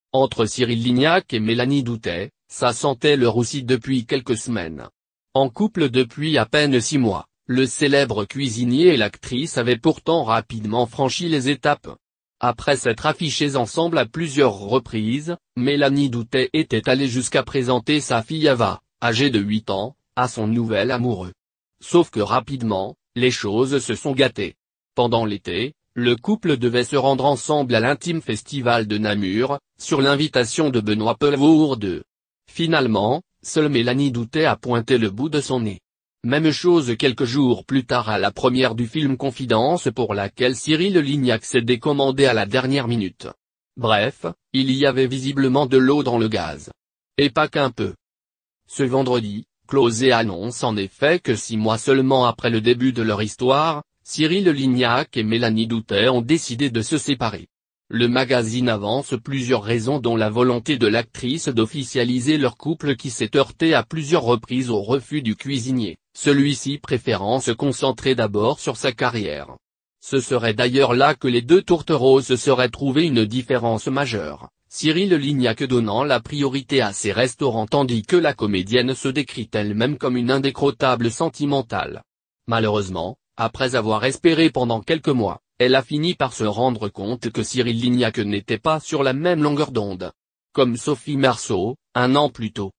Cyril Lignac et Mélanie Doutey : Entre le cuisinier et l’actrice, c’est déjà fini !
Entre Cyril Lignac et Mélanie Doutey, ça sentait le roussi depuis quelques semaines. En couple depuis à peine six mois, le célèbre cuisinier et l’actrice avaient pourtant rapidement franchi les étapes. Après s’être affichés ensemble à plusieurs reprises, Mélanie Doutey était allée jusqu’à présenter sa fille Ava, âgée de 8 ans, à son nouvel amoureux. Sauf que rapidement, les choses se sont gâtées.